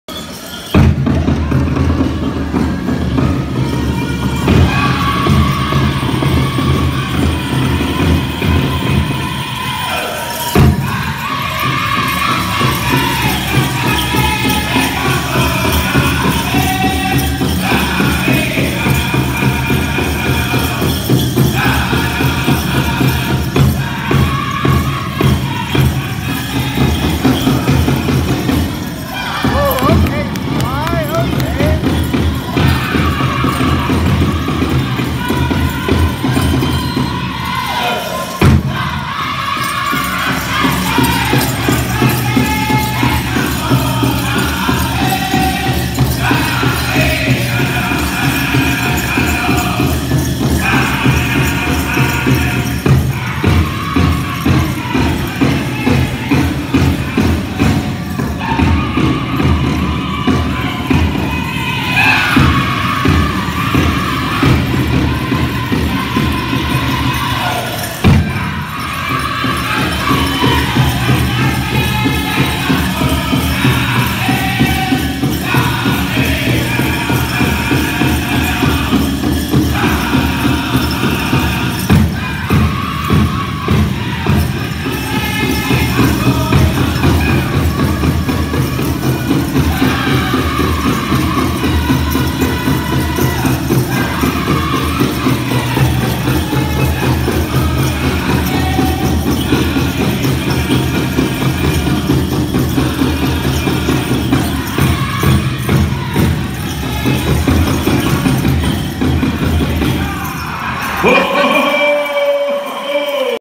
Nothern Cree with a wicked fancy dance song 1st annual elders powwow 2019
Weatherford, Oklahoma.